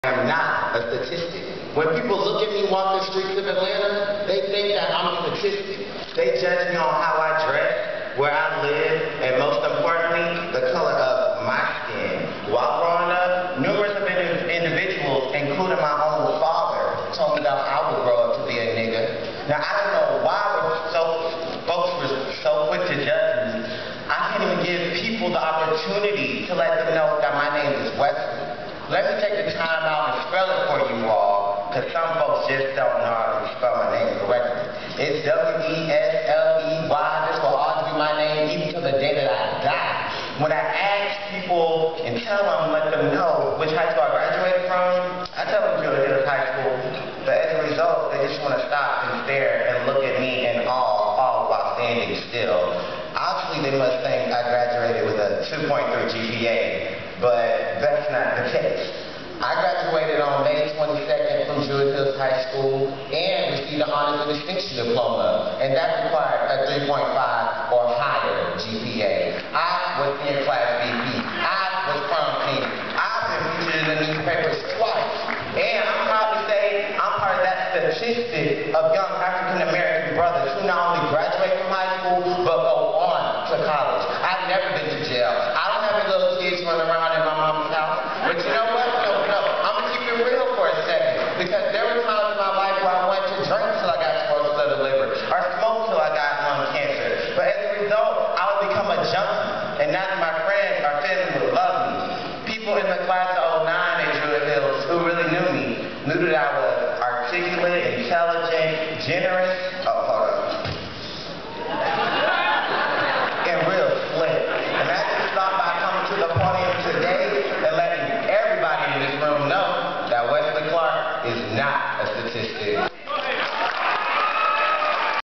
I AM NAS (Z)
Wesley Clark, a 2009 graduate from Druid Hills High School in Atlanta, recited a poem at the 23rd annual 100 Black Men Conference of America in NYC.